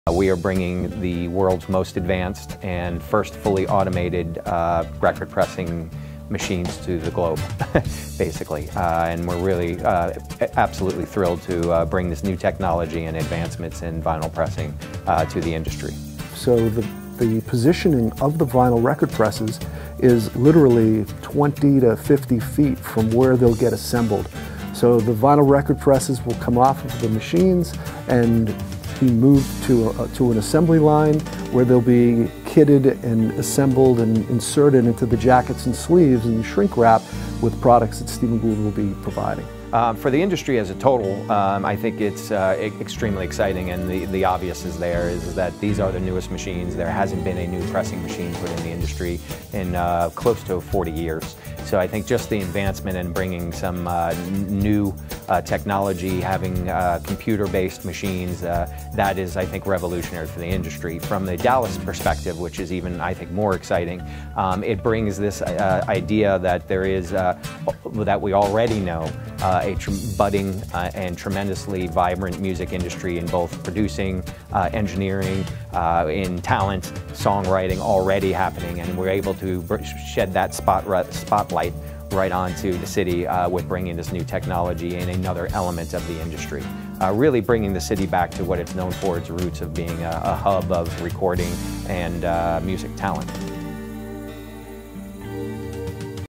Hand Drawn Pressing “Most Advanced Vinyl Pressing Facility in the World”
The first "fully-automated" plant in the world, meaning the computer systems are the newest ones built in over 30 years. It works 4x faster than other plants and makes a perfect record nearly every time. (Benjamin Robinson/The Dallas Morning News)